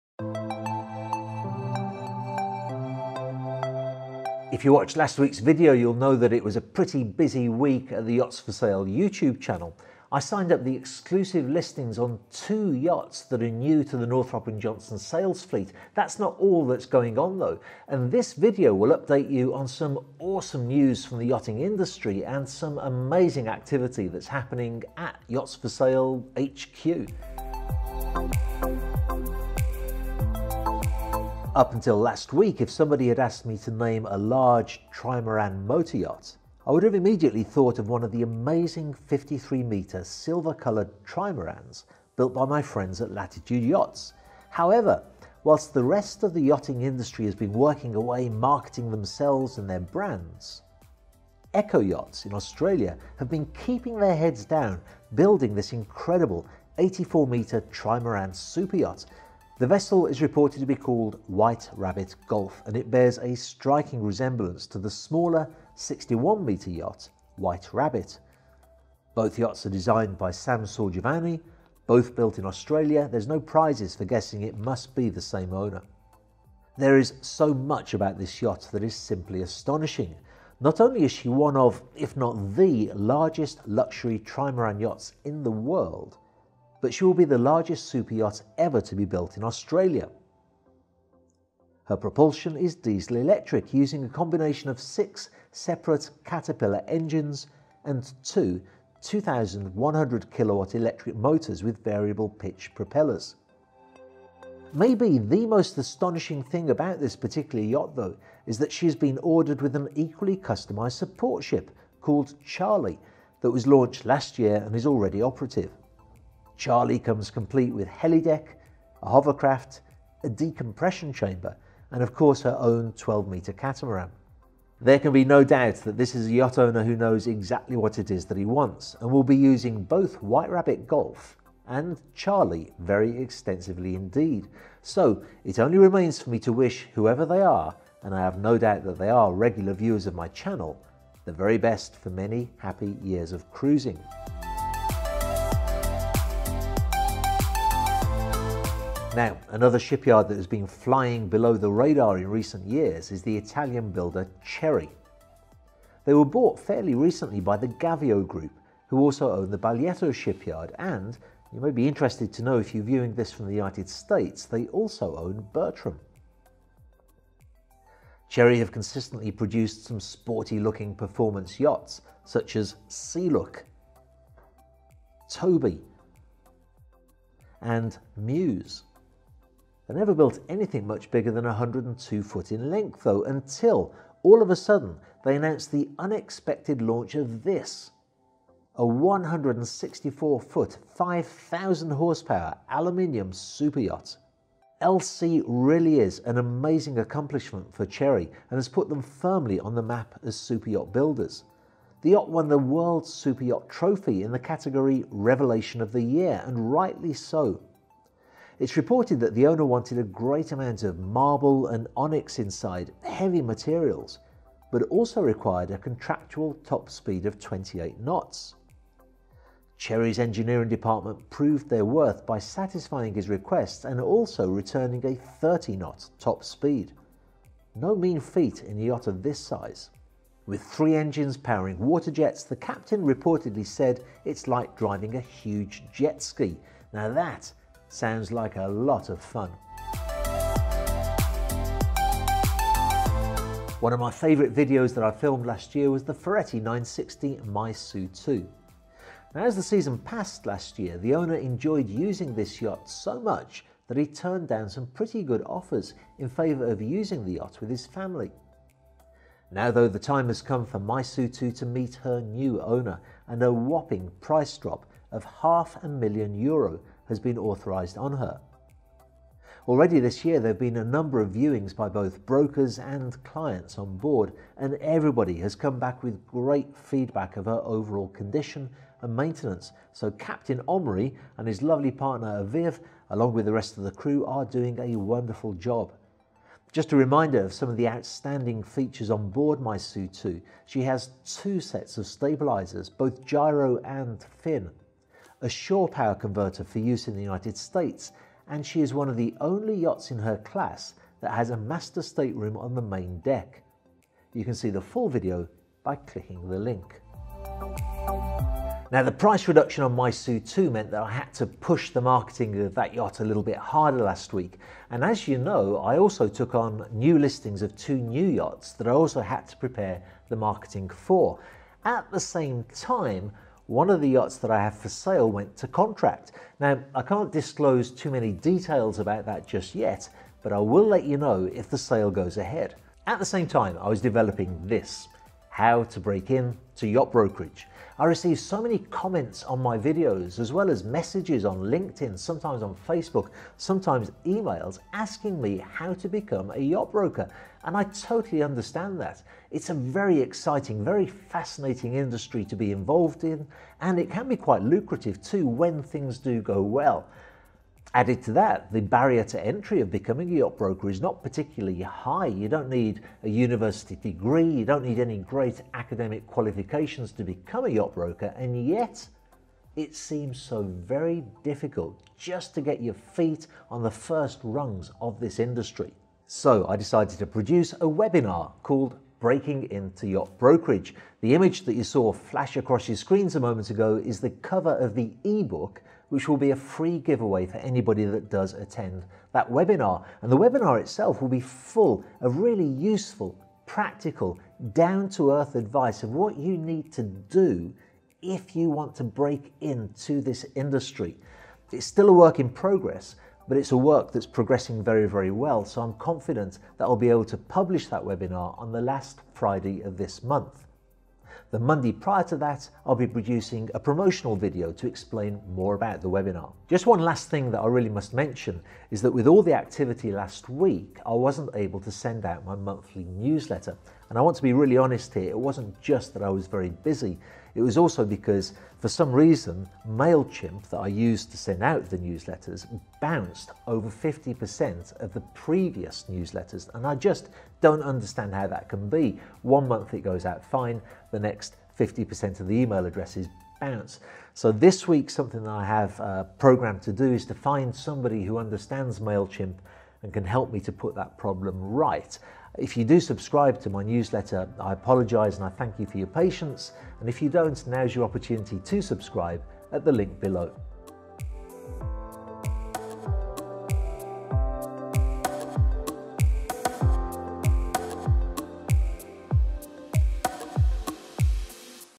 The World's Largest Trimaran Superyacht - and MORE NEWS!
Last week was exceptionally busy, with new listings, price reductions, and developing a webinar called "Breaking in to Yacht Brokerage". While I was occupied with my business though there was plenty going on throughout the yacht industry. This video covers some of the more exciting news items, including the announcement of the world's largest trimaran superyacht, and Cerri yacht's biggest launch to date.